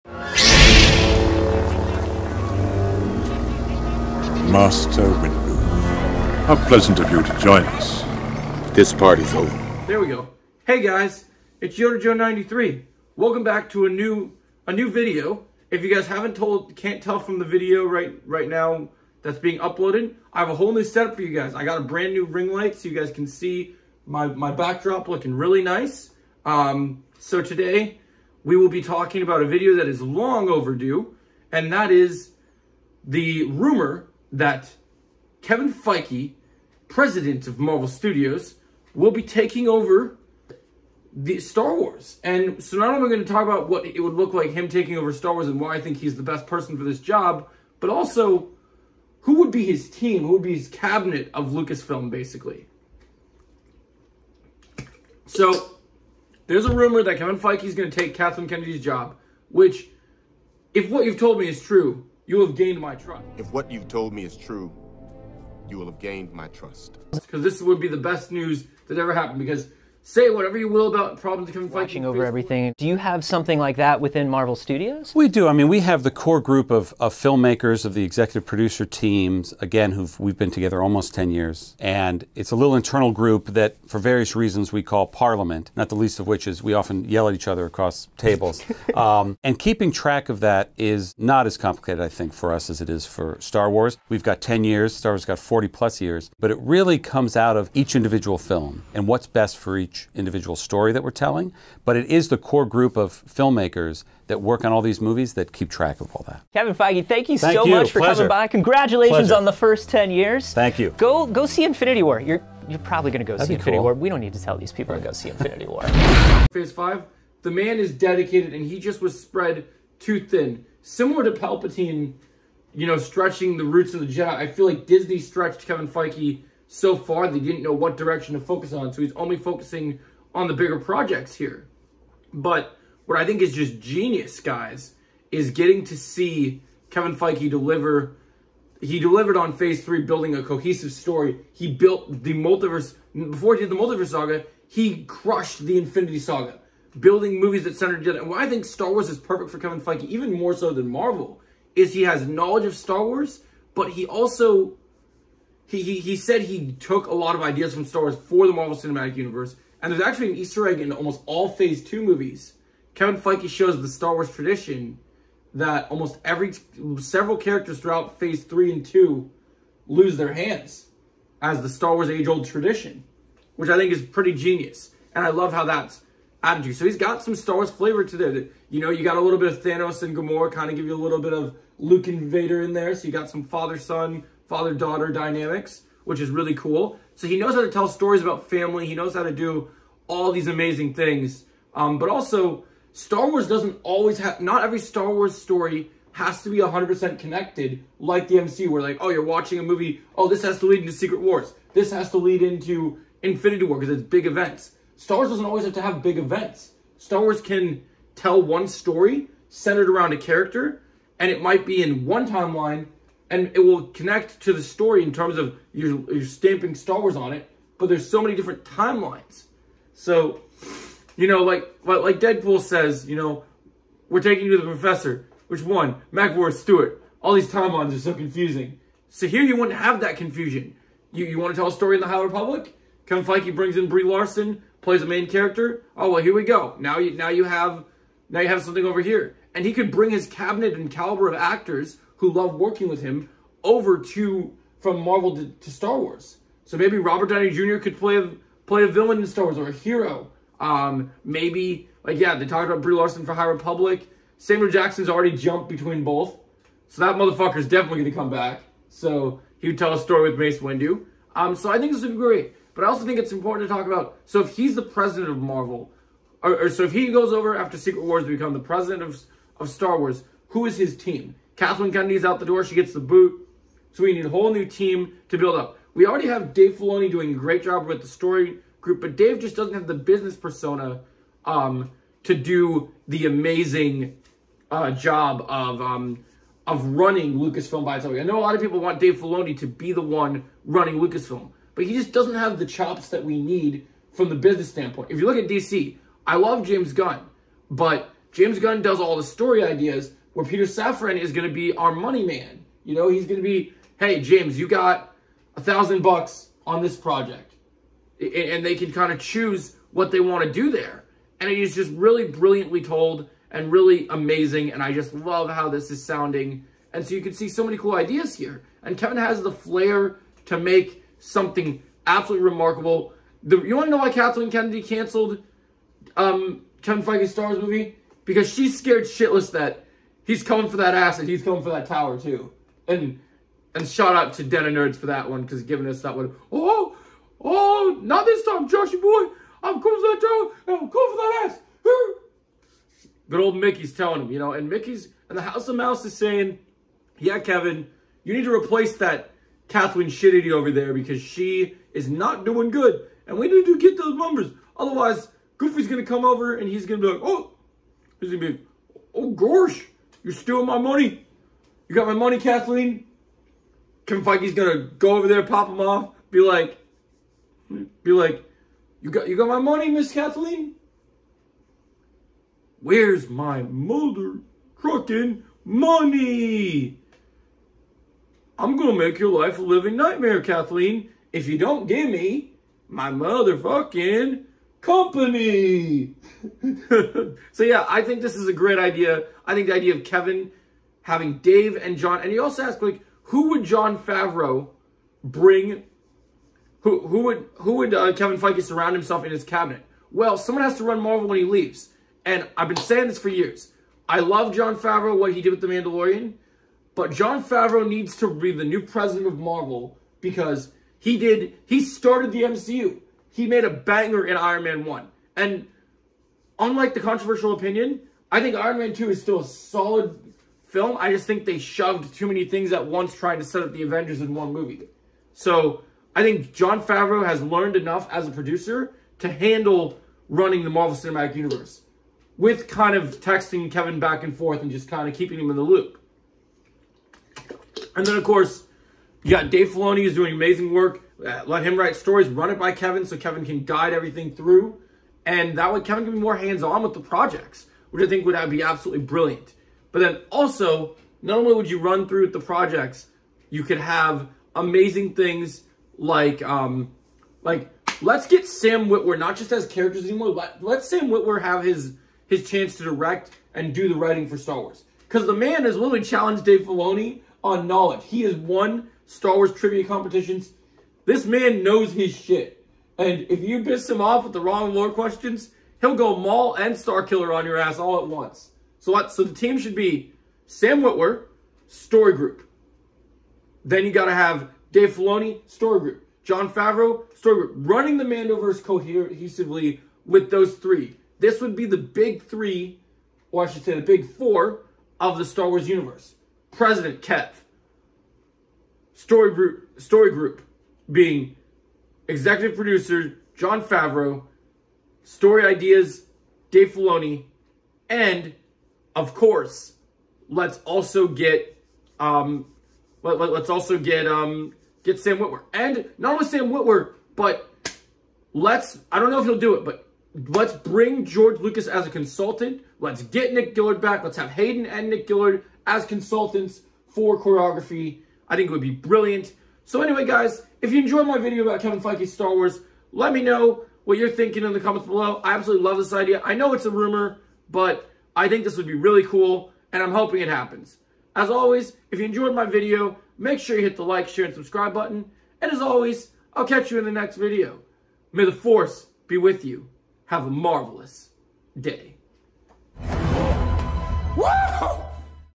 Kevin feige is rumored to take over star-wars
Hey guys it’s Yodajoe93    Today will be talking about. This big rumor that’s been going on for a couple weeks involving Kevin of marvel taking over Star Wars. I personally love this idea and I want to hear everyone’s take.